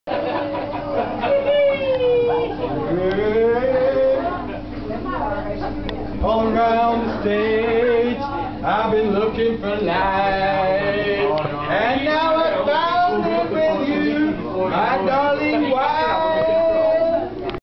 Eamondo dance.avi
Eamondo and his spontaneous hip wriggling, ashtray spinning moves.